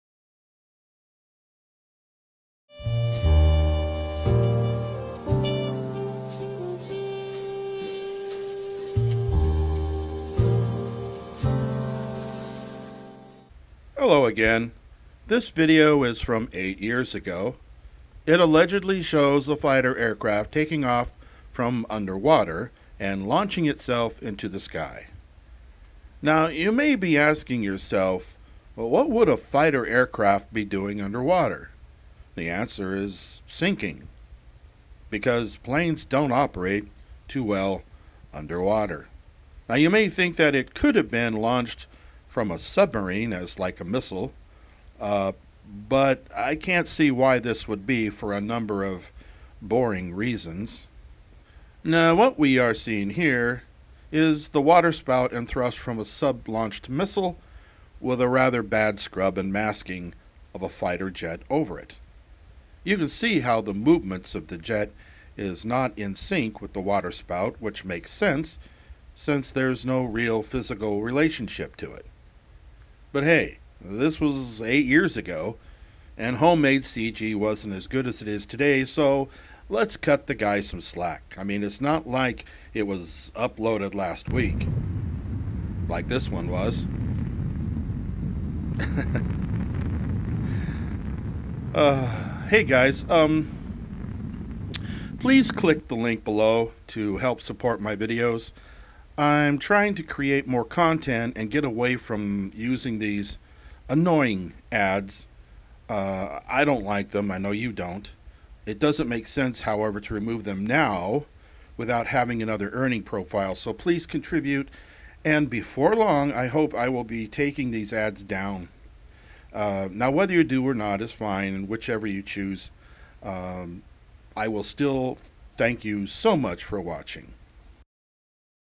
the "Plane Launch From Underwater" Video
I give a report about the plane launch from underwater video